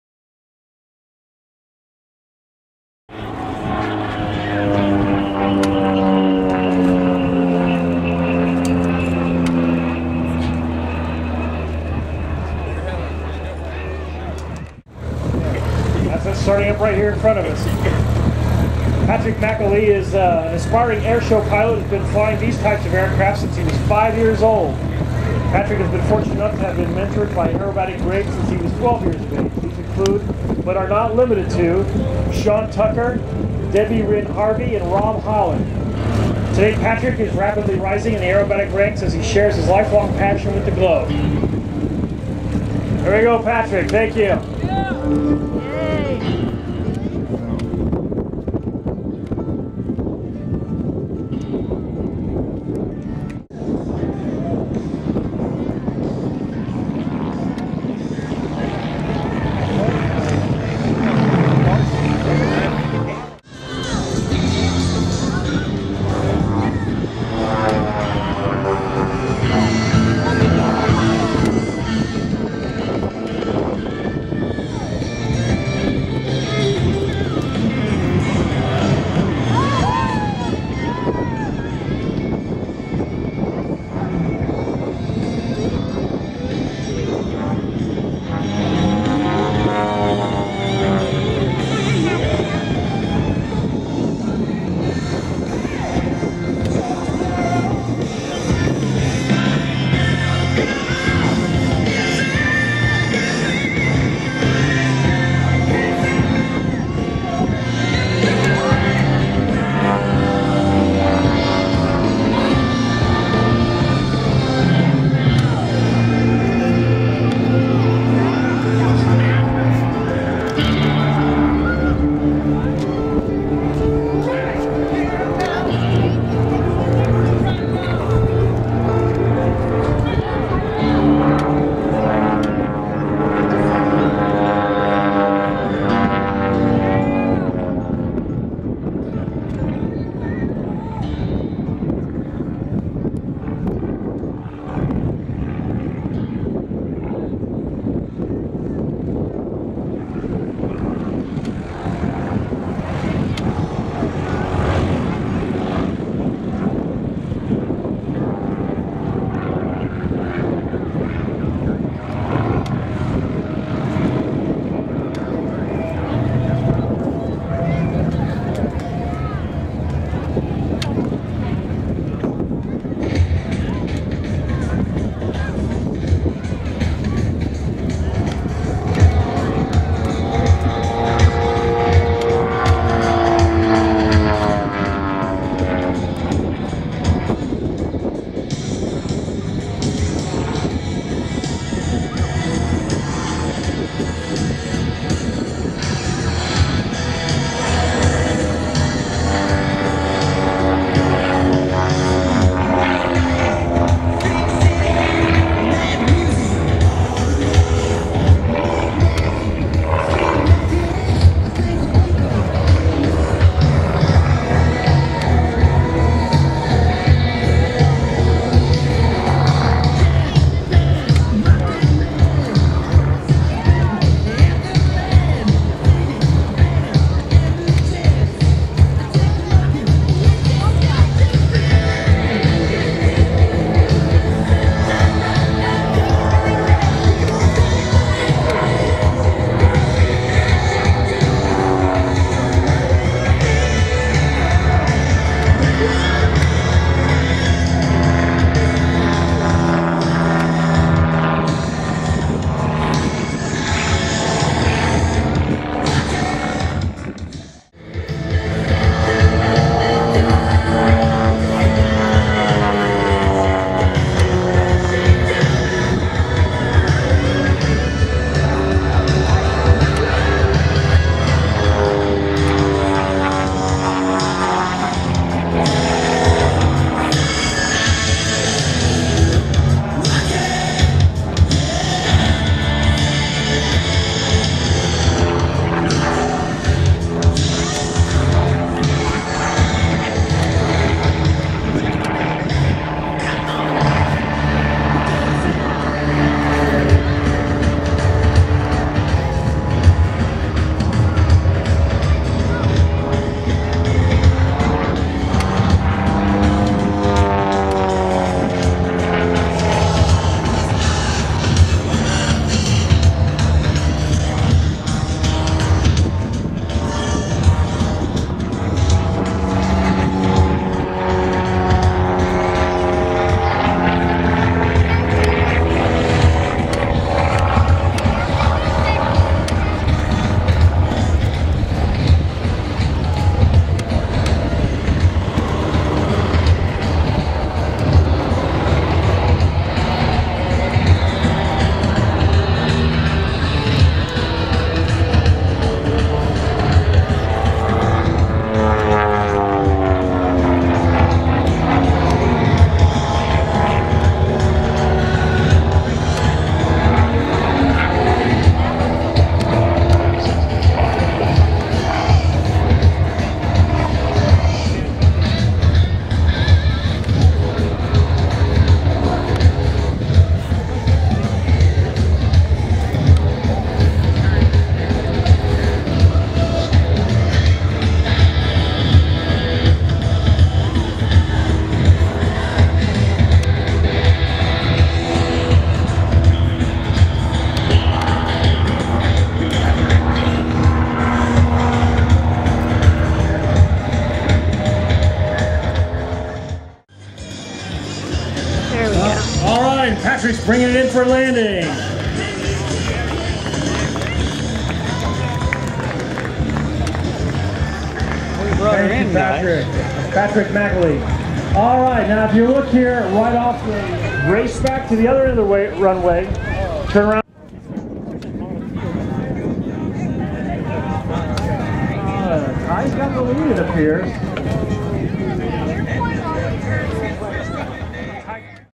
Kokomo Raw Video
Here is the special raw footage of Patricks Kokomo, IN Wings and Wheels 2012 Airshow. Luckily, Patrick flew 2 performances, because of the intense heat, he was having to "ad lib' often to allow for proper cooling do to his intense style. Enjoy and God Bless.